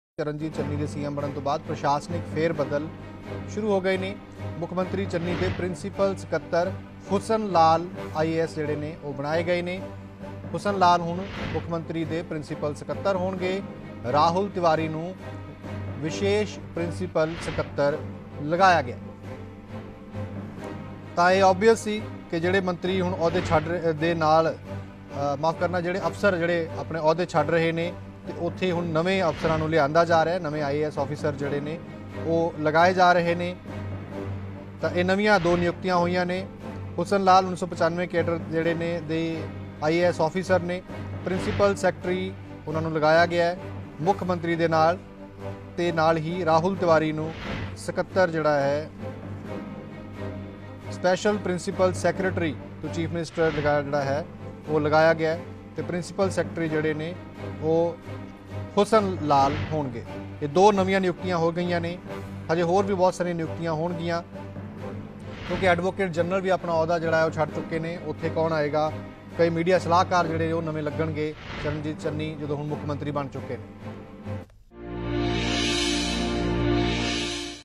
Hussan Lal new Principal Secy, Rahul Tiwari Spl Principal Secy to Punjab CM| Punjab New Cabinet
ਚਰਨਜੀਤ ਚੰਨੀ ਦੇ CM ਬਣਨ ਤੋਂ ਬਾਅਦ ਪ੍ਰਸ਼ਾਸਨਿਕ ਫੇਰਬਦਲ ਸ਼ੁਰੂ
Punjab: Hussan Lal has been appointed the new Principal Secretary to the Chief Minister and Rahul Tiwari the new Special Principal Secretary to the Chief Minister.